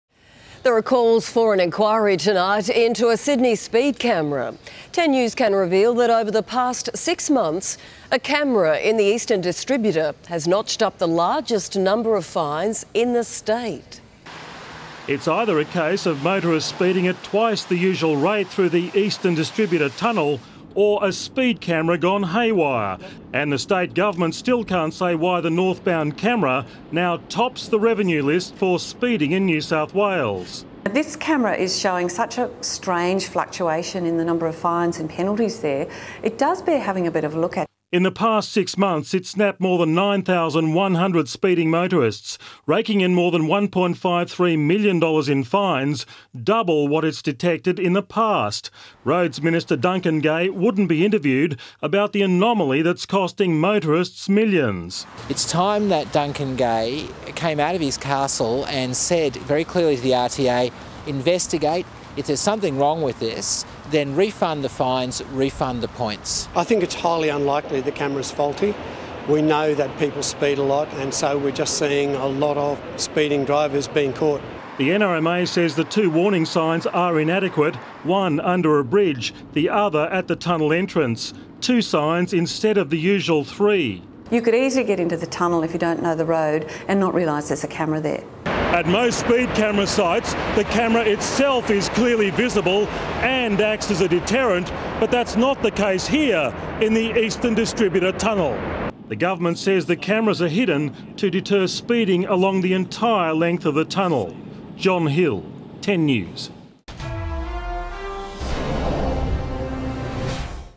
Controversial cameras
There are calls for an inquiry into a Sydney speed camera that's notched up the largest number of fines in the state.